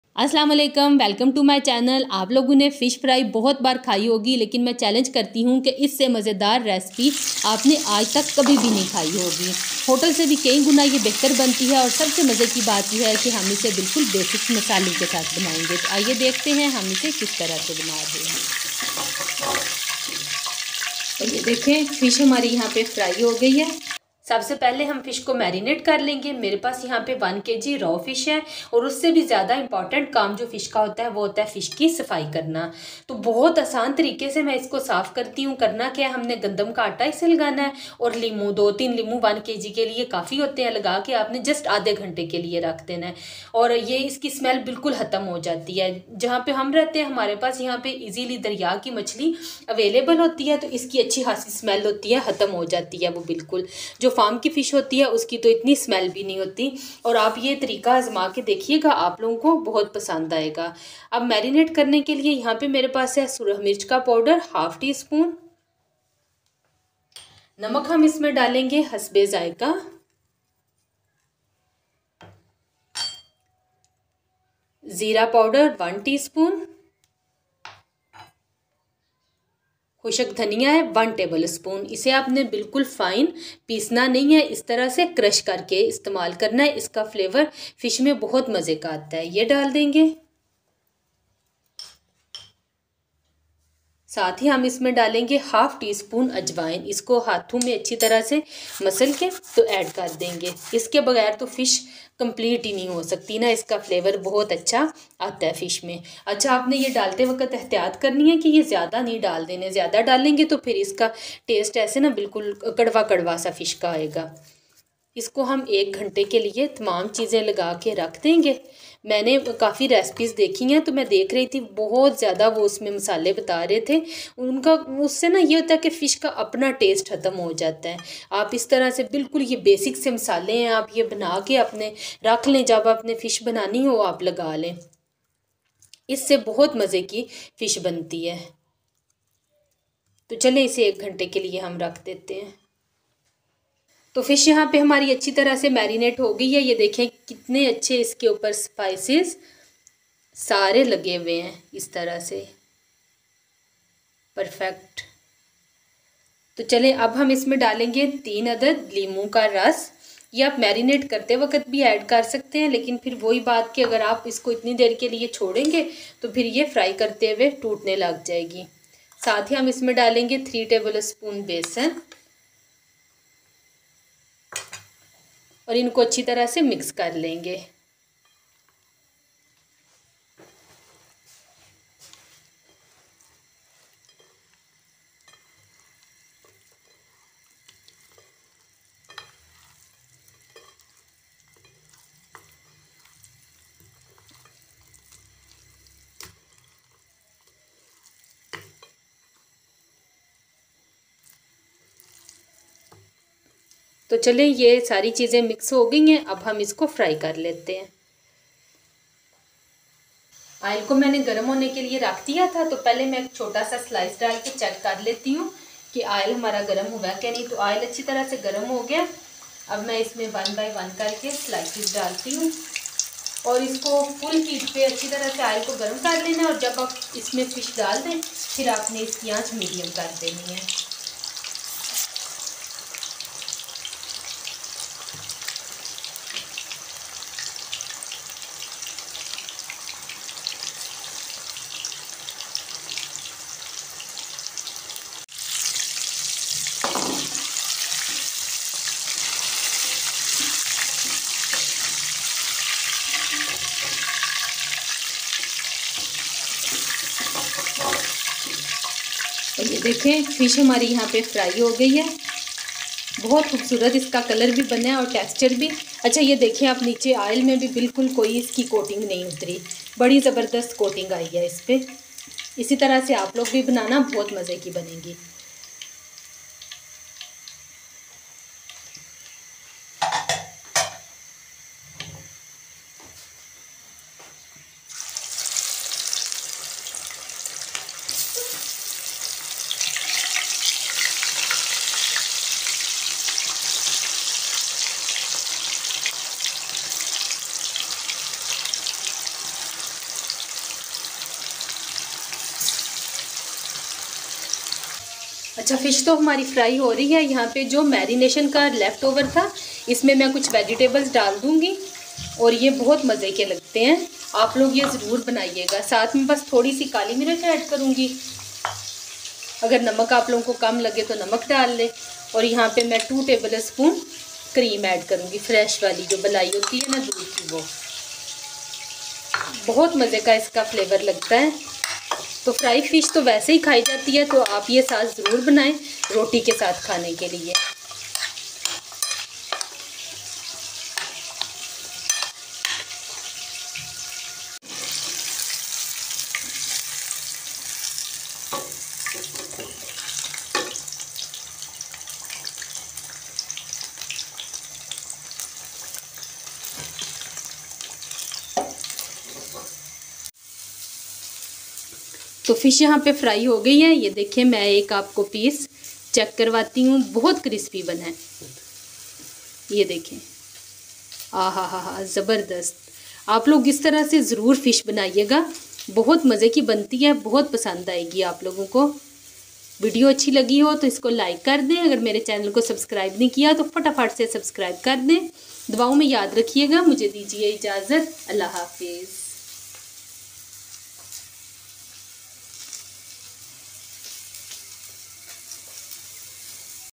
How to make perfect Fish Fry at Home | How to Fry Fish in Urdu/ Hindi | Fish Fry Recipe Pakistani
Winter season is not perfect without enjoying fish fry. And most of the people like to eat it on the banks of lakes, rivers and canals on roadside hotels because of the unique taste found only on those dhabas. But today I will show you how to make fry fish at home in Urdu/ Hindi Pakistani style. And I challenge that it is more tasty than those hotels.